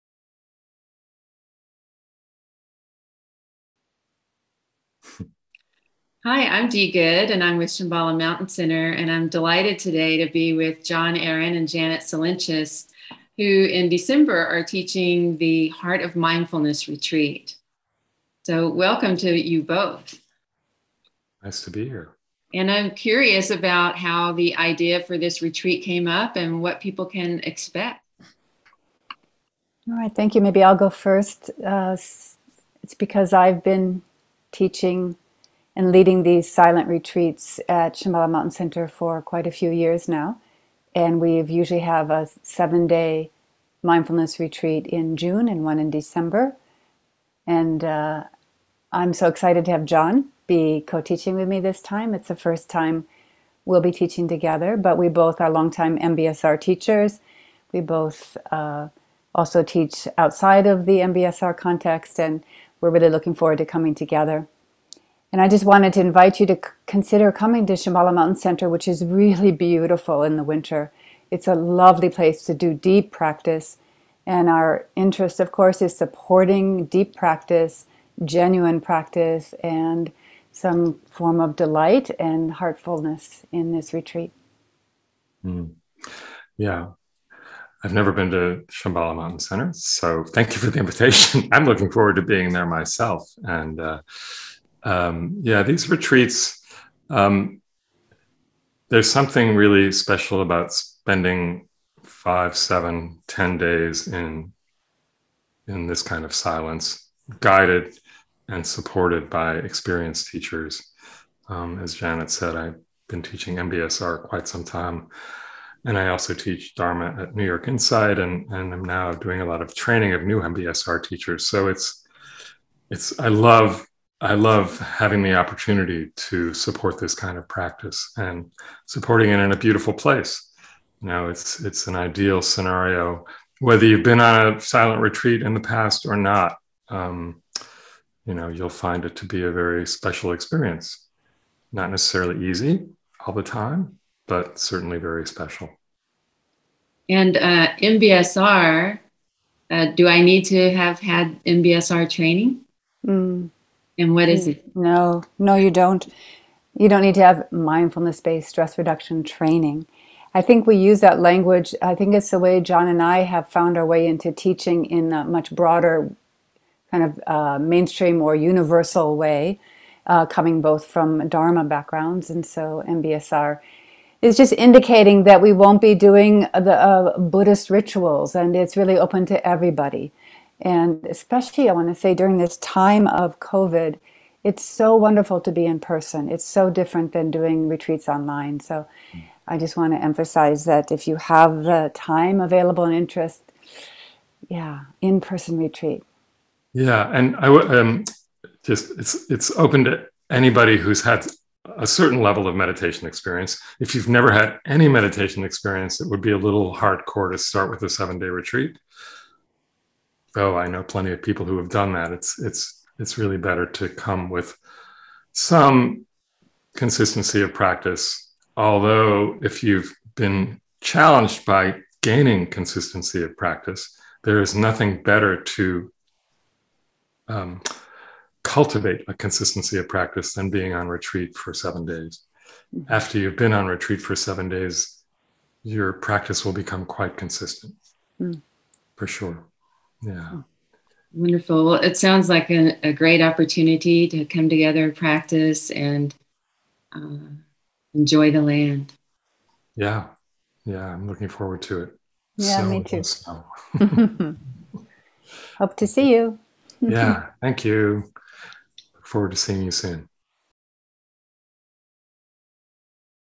The Heart of Mindfulness
Janet Solyntjes and Jon Aaron on their upcoming 7-day silent retreat, The Heart of Mindfulness, at Shambhala Mountain Center in December 2021. Janet and Jon are both experienced MBSR (mindfulness-based stress reduction) instructors as well as dharma teachers. This retreat will draw on secular MBSR techniques. It's not necessary to have a background in MBSR or Buddhism, but some meditation experience is recommended.